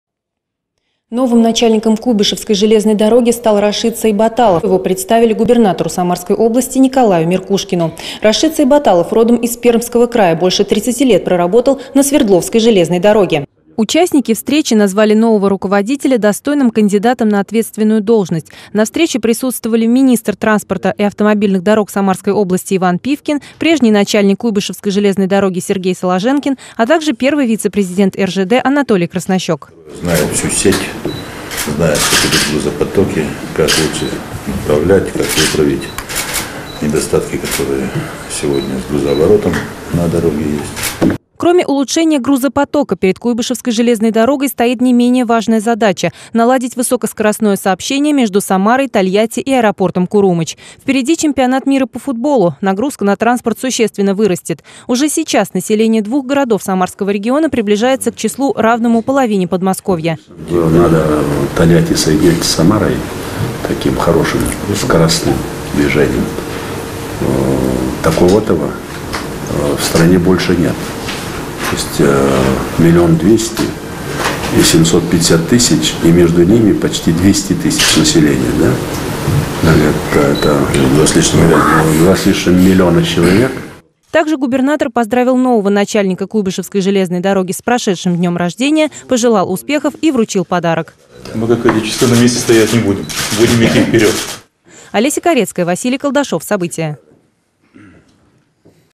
Новый руководитель, новые задачи. Губернатор Николай Меркушкин представил начальника железной дороги
Железная дорога будет иметь ключевое значение для жителей области и не только. Впереди Чемпионат мира по футболу. Об этом глава региона Николай Меркушкин говорил на встрече с новым начальником Куйбышевской железной дороги. Прежний руководитель Сергей Соложенкин переходит на новую должность в Москву.